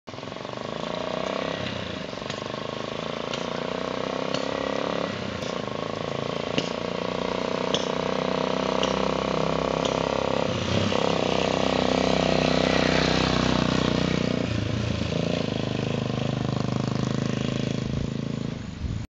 Easy Rider in Sapa, Vietnam
Peter on the saddle of a Honda 125 in Sapa, Vietnam